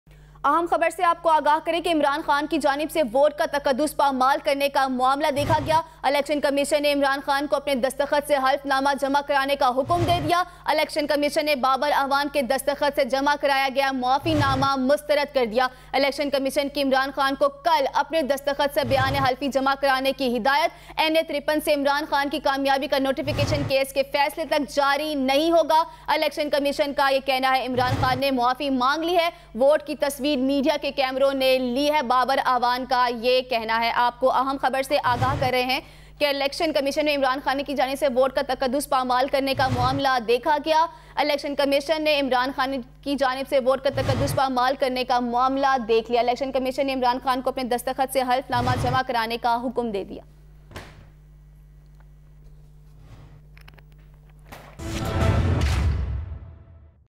CapitalTV; ECP rejects Imran khan's apology affidavit undersigned by Babar Awan
The Election Commission of Pakistan on Thursday ordered Pakistan Tehreek-e-Insaf (PTI) chairman Imran Khan to submit his own undersigned affidavit for violating the electoral code of conduct while casting his vote during the general election 2018.
A four-member bench headed by the chief election commissioner heard the suo motu case against the PTI chief today after the ECP took notice of Imran publicly stamping the ballot paper in NA-53 Islamabad constituency, instead of going behind the voting screen to cast his vote in secrecy.